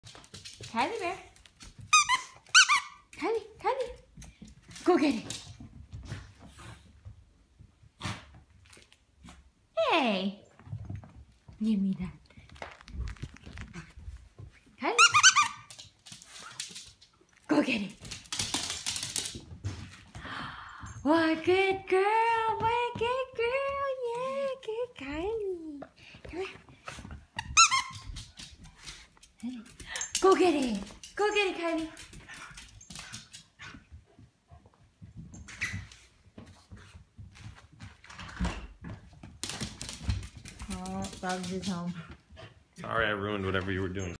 Kylie Bear got an Owl/Penguin for Christmas
My coworker, Alex, was my Secret Santa at work and he got this cool Penguin toy for Kylie Bear. It's really an Owl but it looks like a Penguin.